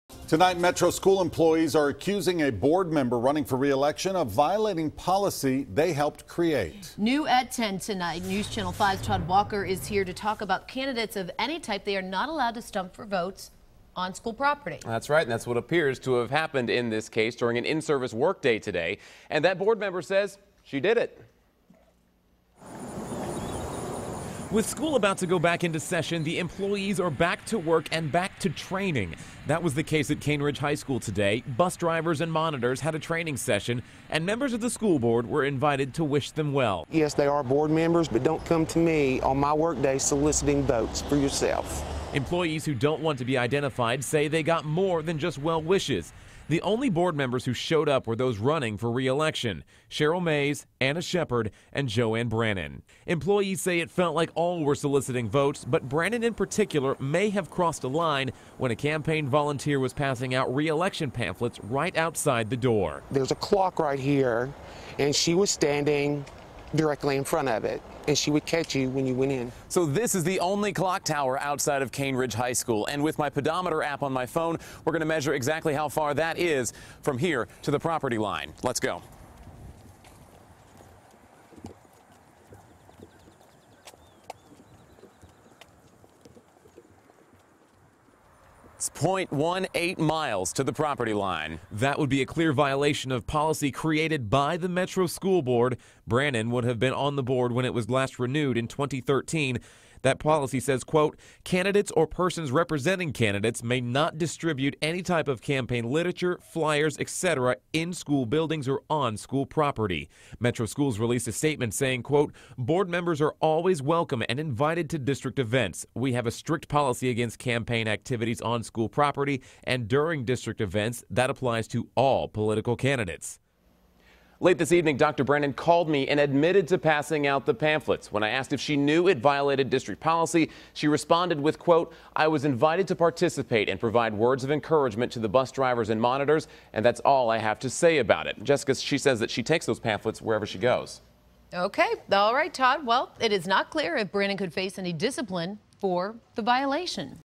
Metro School Board Member Appears To Violate Own Campaign Policy
Metro Schools employees are accusing a board member running for re-election of violating policy they helped create.